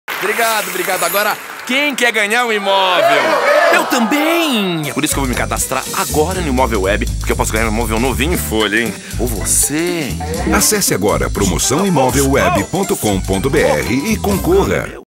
Promoção "Meu Imovelweb"
Participe e concorra a um imóvel! É só fazer o cadastro e torcer! E toda semana, até 3 chances extras para participar!